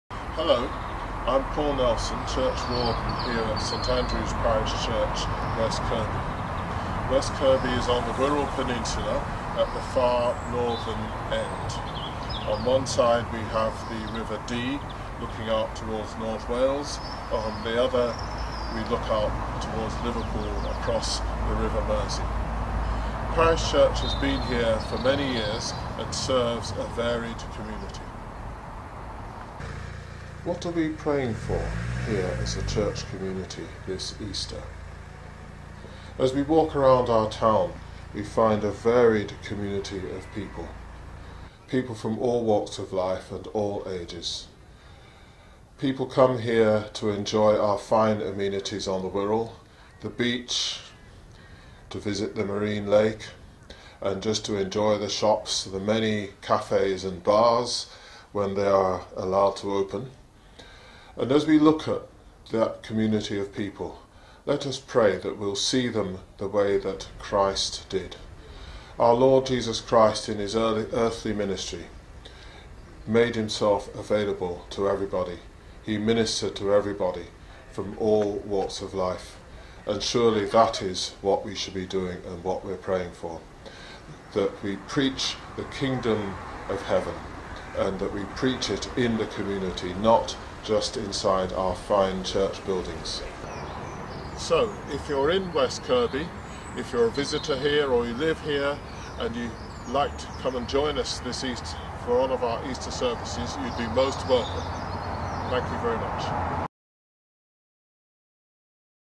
TCK 2021, Paul Nelson, Churchwarden at St Andrew's West Kirby
Paul Nelson, Churchwarden at St Andrew's parish church, West Kirby, offers a prayer in the lead up to Thy Kingdom Come.

Thy Kingdom Come is a global initiative, in which Christians are called to pray from Ascension to Pentecost for more people to come to know Jesus. This year it runs from Thursday 13 to Sunday 23 May.

The Diocese of Chester is encouraging everyone to participate. Whether you are new to prayer or it has been your lifelong discipline, there are resources to help you participate individually or as part of your family or church.